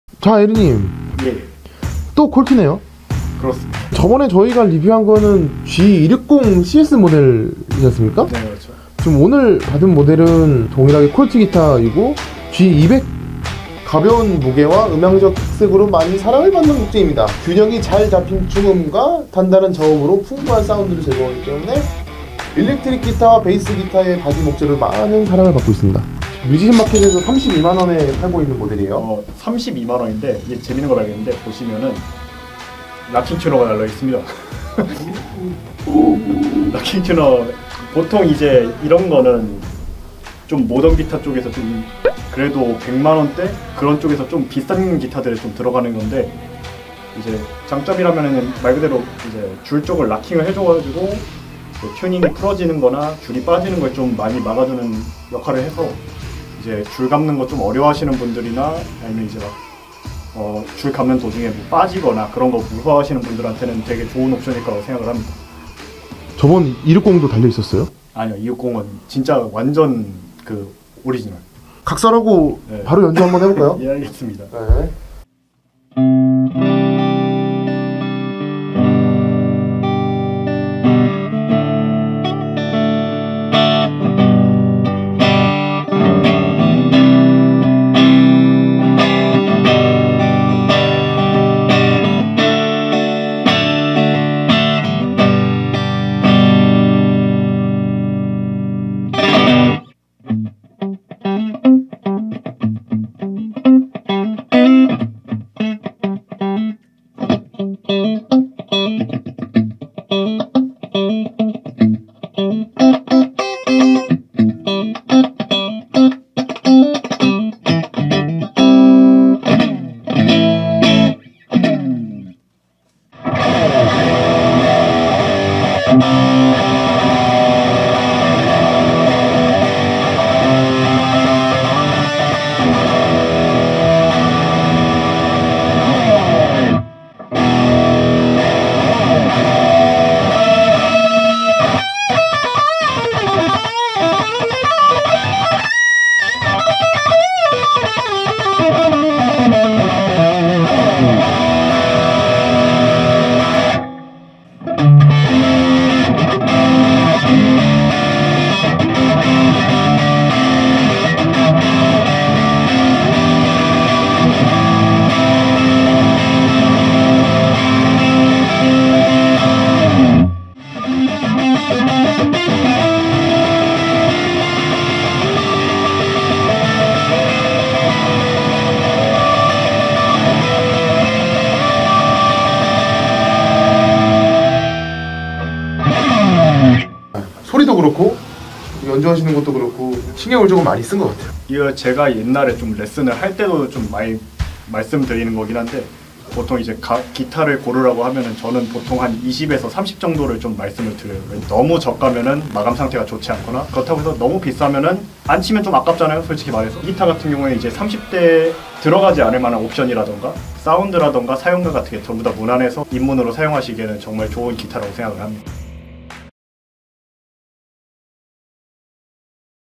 30만원대 기타에 이게 달려있다고?!!!!!? | 콜트 G200 기타리뷰
안녕하세요 ! 이지두밴드입니다!
뮤지션 마켓 리뷰 네번째 컨텐츠 입니다!
이번에도 국산기타의 거장 콜트의 입문자용 라인 G200 기타를 에르님과 리뷰해봤습니다.

참고사항으로는 저희가 리뷰한 모델 색은 판매를 안하는것 같습니다
분홍색 민트색 검정색 이렇게 세가지가 선택 가능하니 참고하여 주세요 ^^

오늘도 잘 부탁드립니다!

콜트 G200 구매링크 [해당링크 통해 구매 시 5% 할인 금액으로 이용 가능합니다!]
무려 인터넷 최저가보다 저렴한 304,000원 ㄷㄷ
30만원대라고 이야기 했지만 30만원에 4천원 더 얹어서 아주 저렴하게 좋은기타 살 수 있는 기회라고 생각합니다 ^^